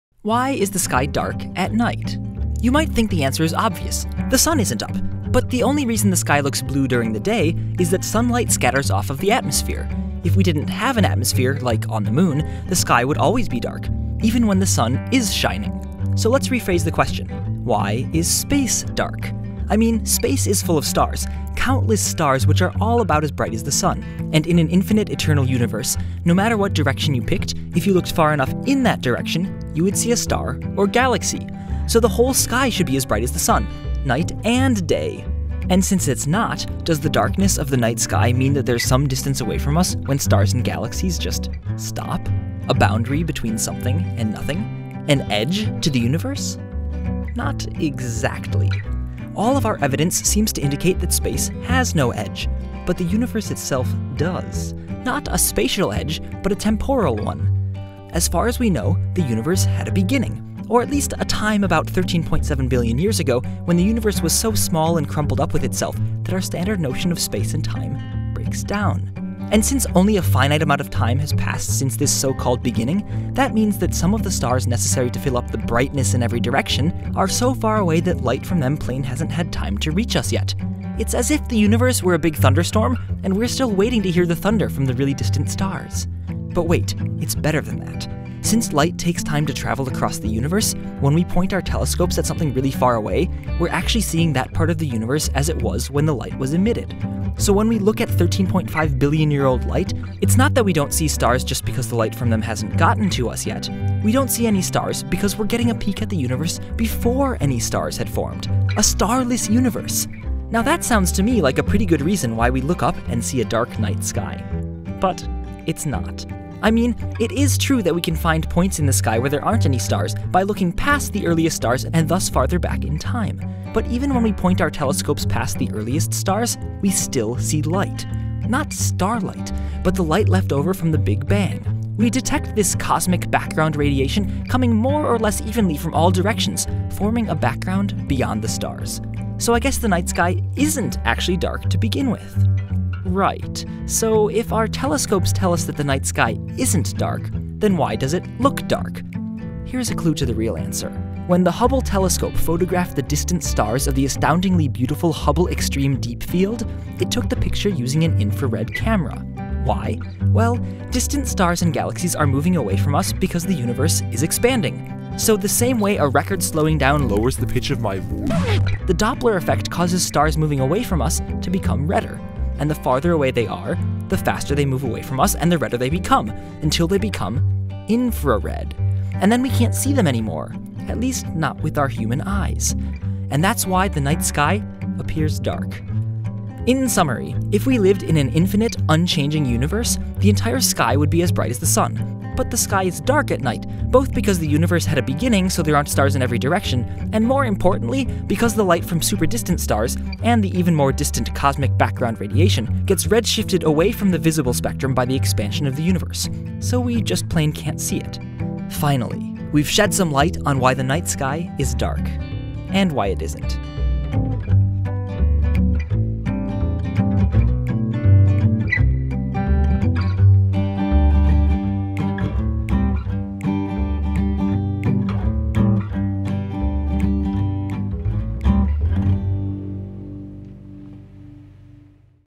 Why is it Dark at Night?
Have you ever wondered why you look up and see a dark sky at night?

Thanks to Nima Doroud and Niayesh Afshordi for contributions and to Perimeter Institute for support.

Created by Henry Reich Created by Henry Reich